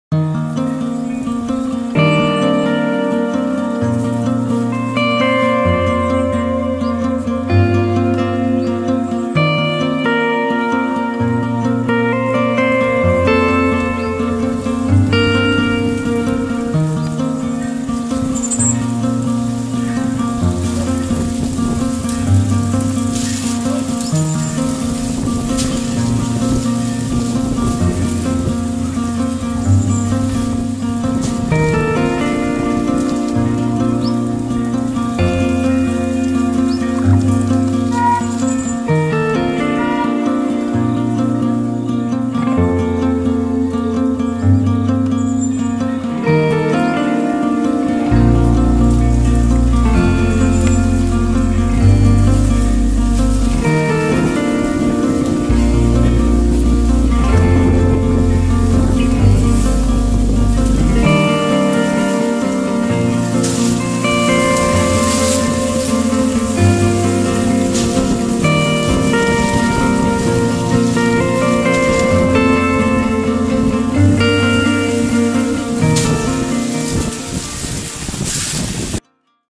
Cigueñuela común, Focha común, Chorlitejo grande, Gallineta común
Arucas, Gran Canaria. Junio 2021